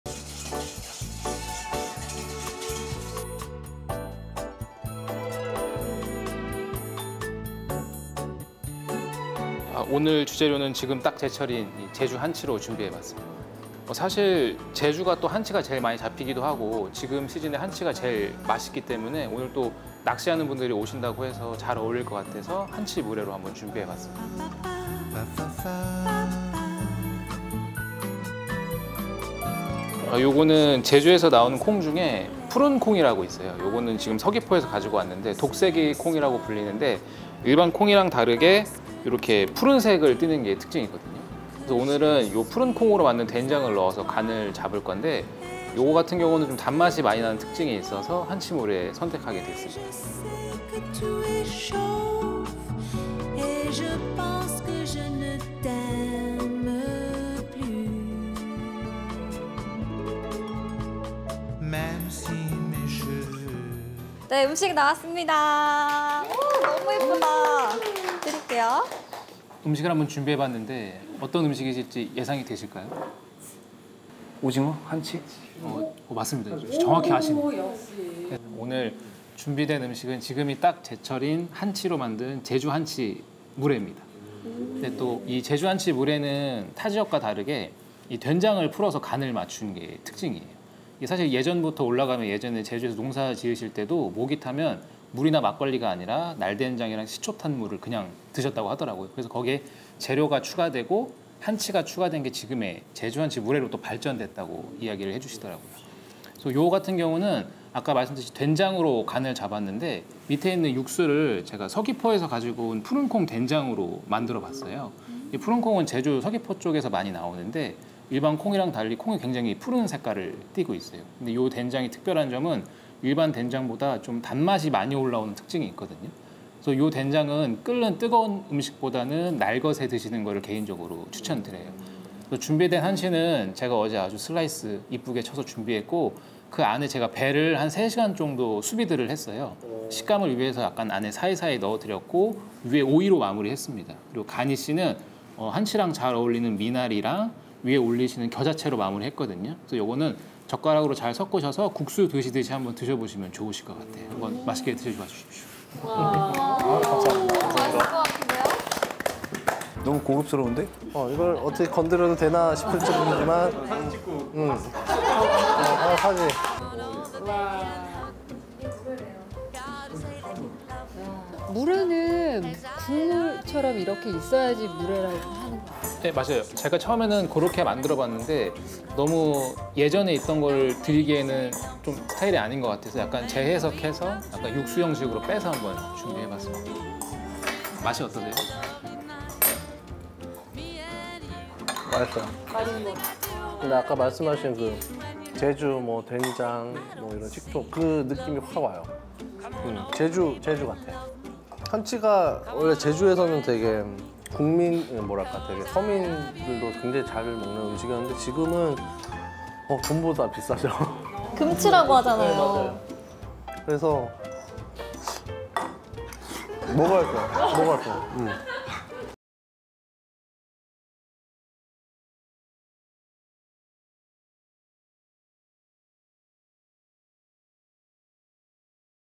[핫!클립] 흑백요리사 출연 흑수저 'B급셰프'의 'A급요리' - 다이닝식, 제주한치물회
전세계를 강타한 요리경연 프로그램 흑백요리사!
80인의 흑수정 요리사 중 1인.
B급셰프 강병욱 셰프

비록 1차전 탈락의 고배를 마셨지만,
제주의 대표 셰프로 활동하고 있다.

흑백요리사에서는 미쳐 보지 못한
B급셰프의 A급요리!
제주MBC만이 보유한 강병욱 셰프의 요리를 소개합니다.